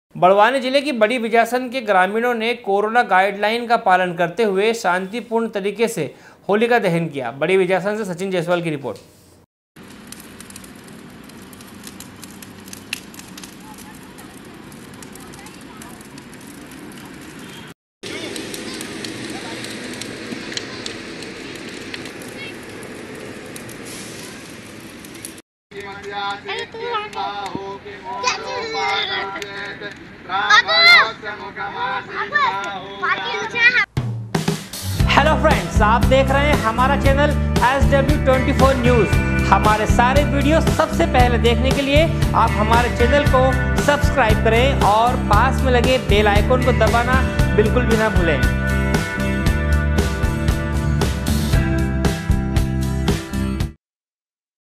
badi_bijasan- बड़वानी जिले की बड़ी बिजासन के ग्रामीणो ने कोरोना गाइडलाइन का पालन करतें हुए
बड़वानी जिले की बड़ी बिजासन के ग्रामीणो ने कोरोना गाइडलाइन का पालन करतें हुए शांतिपूर्ण तरीके से होली का दहन किया। बड़ी बिजासन से सचिन जायसवाल की रिर्पोट।